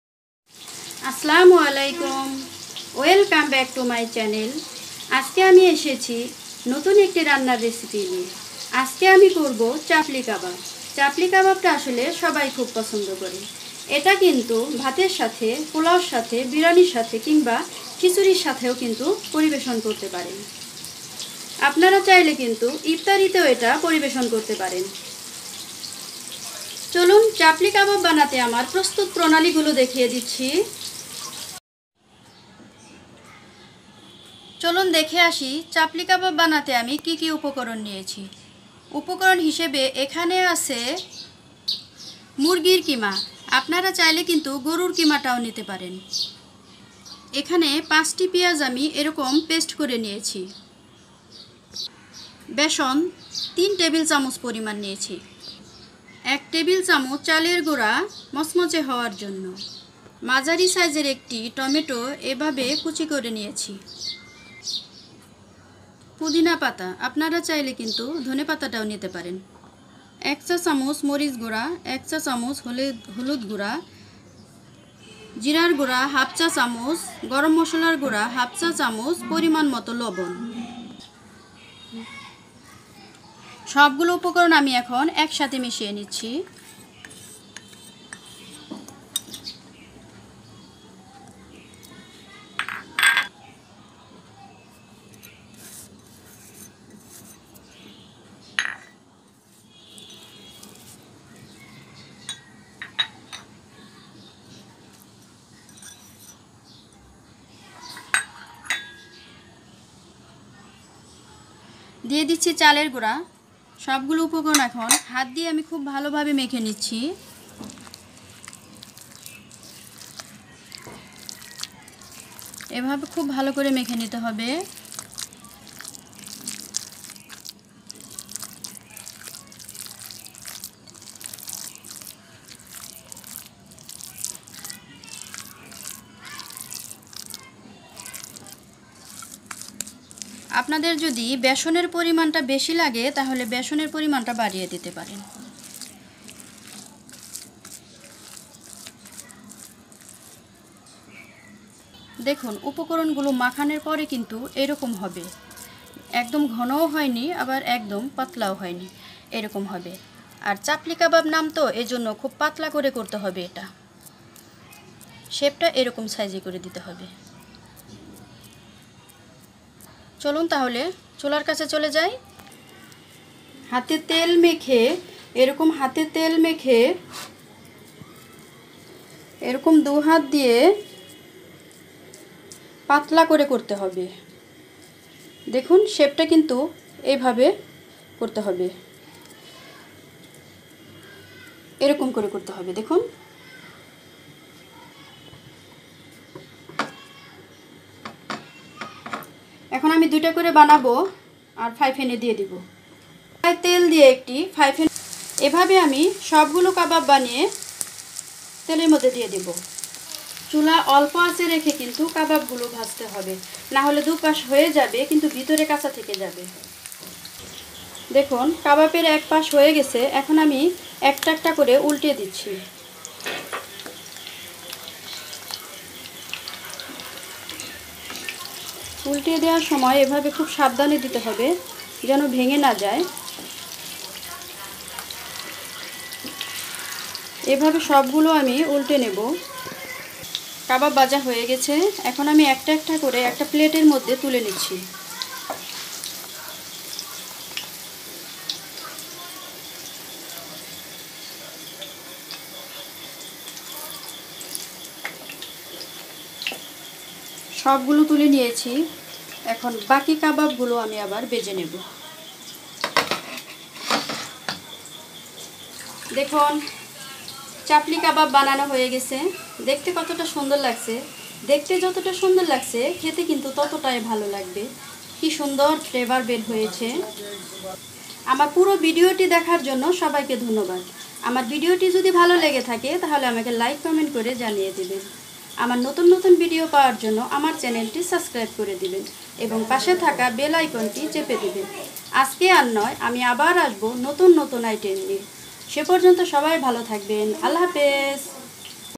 চিকেন চাপলি কাবাব রেসেপি। কিভাবে চাপলি কাবাব তৈরি করতে হয়|How to make kabab| Bangladeshi kabab
In  this video we gonna talk about  how to make Capli Kabab. How to make kabab. How to make Bangladeshi kabab.Tasty  Bangladeshi kabab|চিকেন চাপলি কাবাব রেসেপি। কিভাবে চাপলি কাবাব তৈরি করতে হয়|